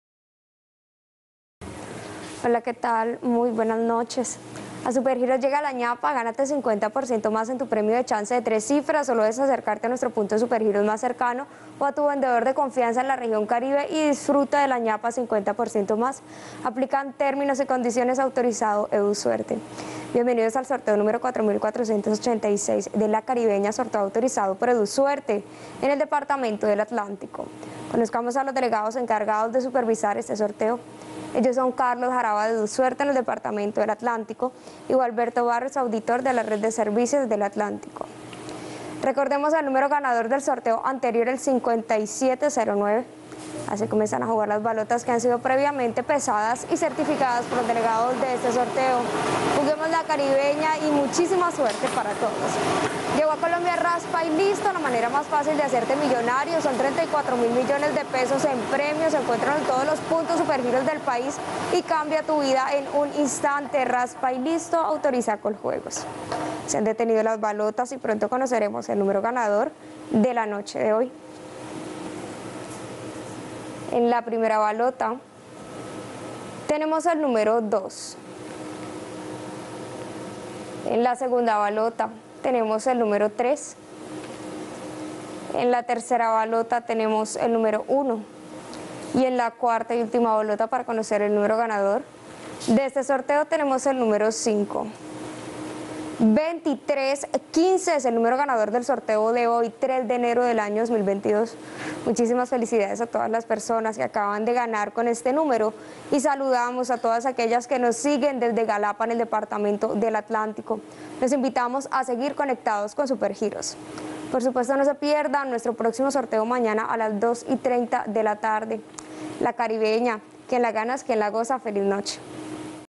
Resultados CARIBEÑA NOCHE Lunes 03 de enero de 2022 ✅🥇🔥💰
Hola buenas noches, te presentamos los resultados de las loterías y chances en Colombia, vamos con el Resultados del Chance Caribeña Noche del Lunes 03 de enero de 2022.

Resultado LA CARIBEÑA NOCHE Del Lunes 03 de enero